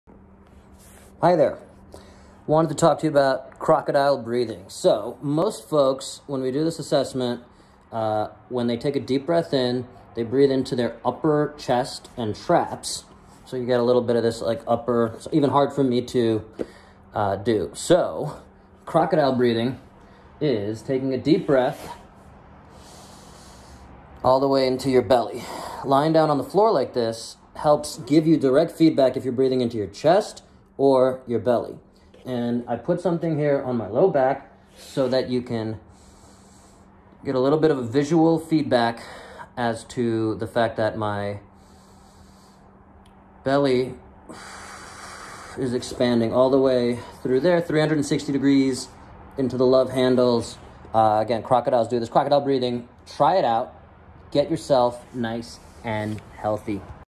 Crocodile breathing
Coming to you LIVE (at some point)... from SoCal
Reminder to get your crocodile breathing in 🌬🌬🌬
There is no one simple way to improve your breathing... it should be a practice, a pursuit. Spend 3 minutes on the floor working on this and be a better human.